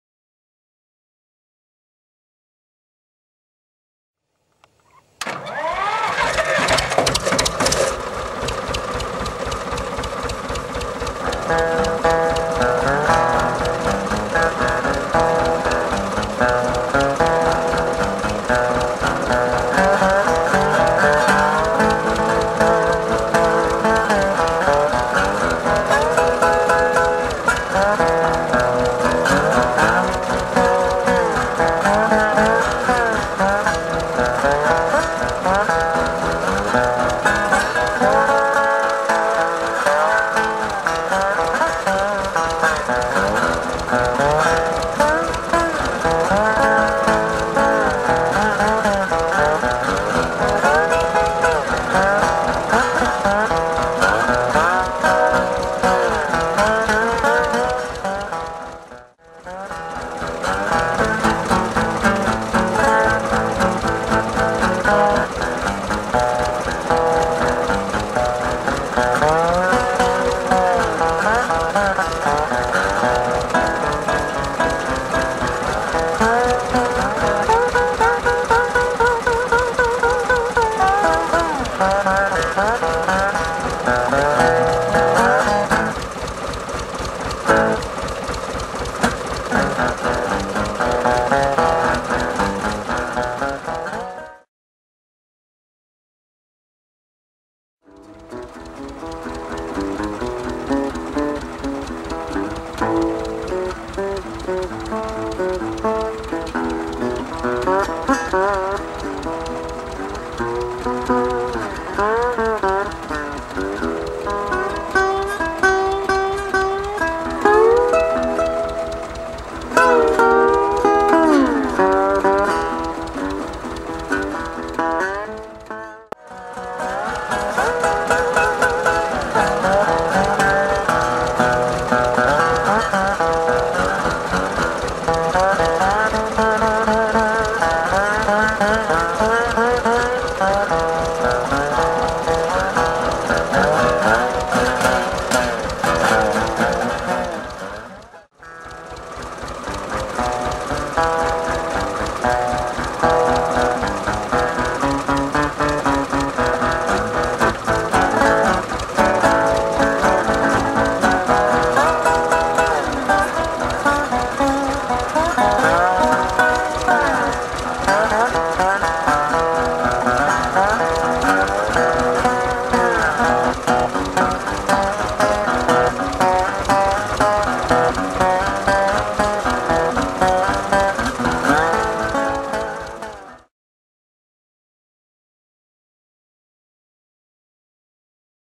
Delta Tractor Blues Jam - 1933 National Duolian & Victor the tractor..
Ol'Rusty, 1933 Duolian meets the Old school drum machine!!!
Had a GREAT time jammin' here, a bit tricky since the pace goes up and down and this was my FIRST ever TEST to see how it would work out. Sitting too close to the engine itself you I learned that you tend to not hear the boff-boff sound from the exhaust pipe..So yes, we're in and out of time here and there.
This is a jam featuring an old tractor, a Bolinder-Munktell model VICTOR!
A two-cylinder Swedish made farm hand, great for plowing AND for playing the blues.. LoL!!
Thanks to Bo-Lennart Eriksson for letting me borrow the BM tractor for a while..!!